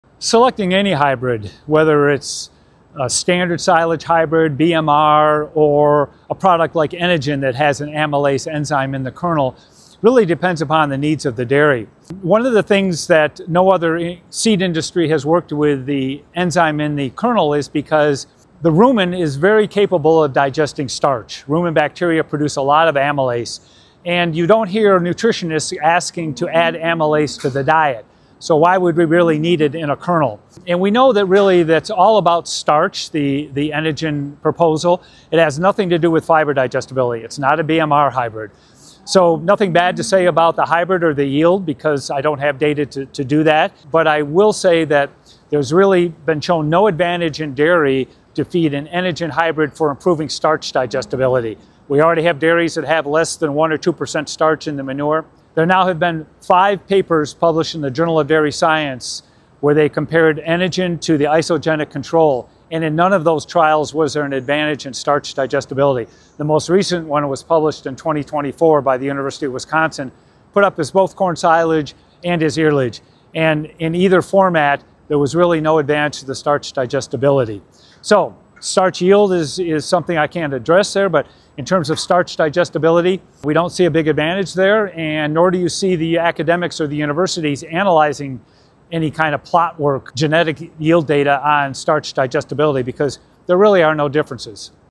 Does adding amylase to a silage hybrid improve starch digestibility? (No)
Does adding amylase to a silage hybrid improve starch digestibility? Research says no.

🔬 5 studies found no advantage.
🐄 The rumen naturally produces amylase, so added enzymes aren’t needed.
🌽 Starch digestibility? No difference.

Stick with proven hybrids. TonnEdge delivers.